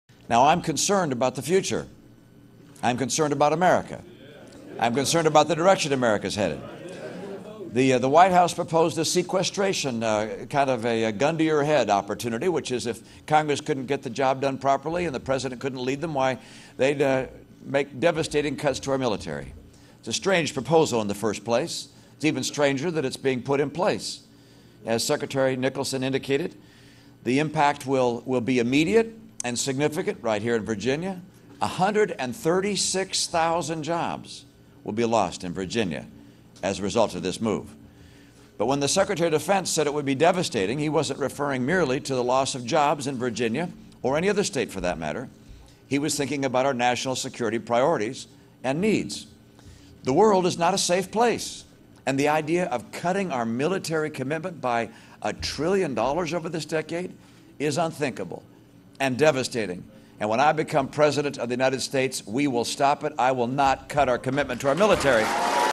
Romney Campaigns in Virginia
GOP presidential nominee campaigned in Springfield, Virginia Thursday. At an American Legion post, Romney said President Barack Obama isn't leading Congress in the face of what he called devastating military cuts. (Sept. 27)